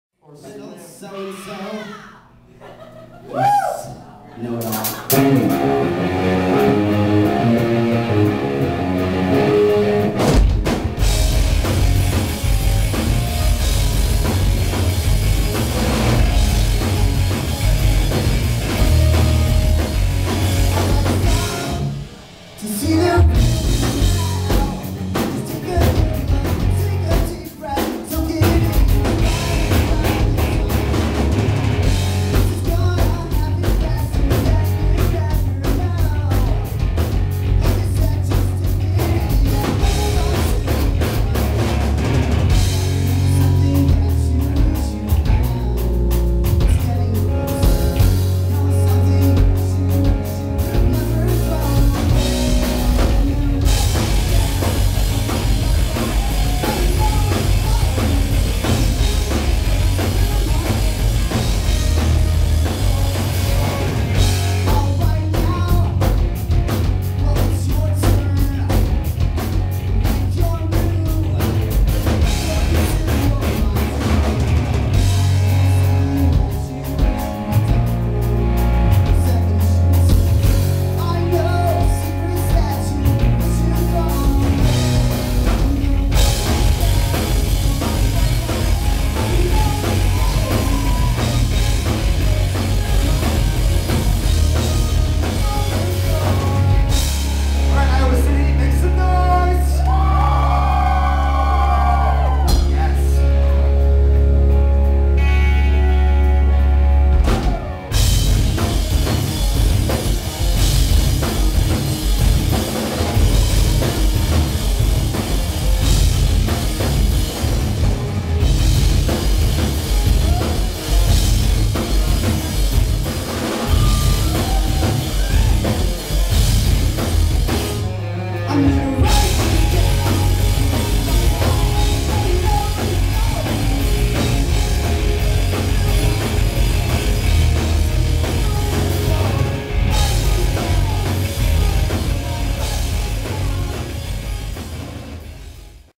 Danger Is My Middle Name @ The Picador on September 17th #3
Danger Is My Middle Name performs live at The Picador in Iowa City, IA on September 17th!